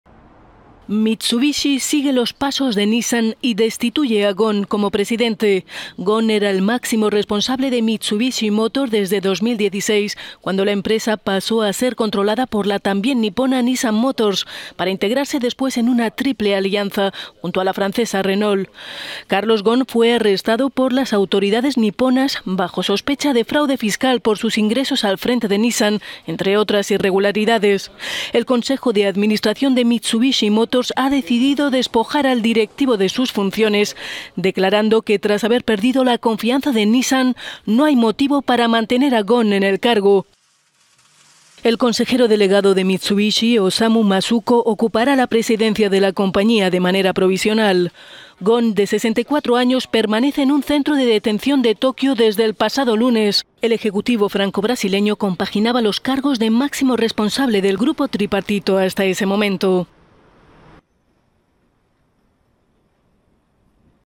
Mitsubishi también destituye a Ghosn como presidente
Mitsubishi, al igual que Nissan, destituye a Ghosn como presidente…